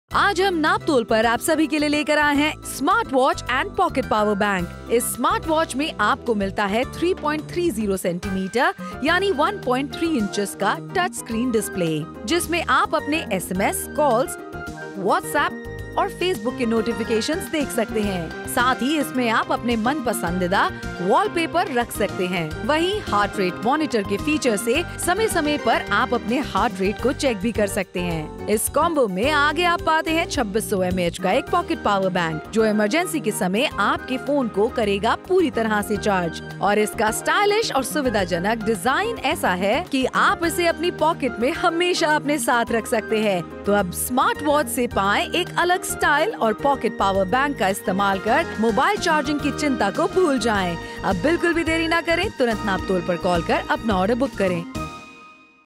Smart Watch with Free Pocket Powerbank ID116 (Code:11571)
Smart watch for smart people

This Smartwatch offers several features that can make life more convenient, such as notifications, fitness tracking, and voice control. Additionally, this power bank is useful for keeping your fitness devices, such as fitness trackers or headphones, charged and ready to use.

Highlights:
Trendy smart watch with notifications
Great for people of all ages
Value for money deal
Pocket Power bank
Great for gifting

Features:
Coloured display
Touch sensor
Heart rate monitor (accuracy 85%)
Bluetooth notification
Direct charging
Call notification
SMS notification
Customized wallpaper
Social media notification
Splash resistance (water drop resistance)
Music controller
Activity monitor
Emergency powerbank charger

Specifications:
Screen: 3.3 cm display
Type: Capacitive touch button
Bluetooth: 4.2 A2DP
Music Player: Control by your mobile
Power bank: 2600 mAh can charge this watch 10 times

Package Contents:
1 x Smart Watch
1 x Pocket Powerbank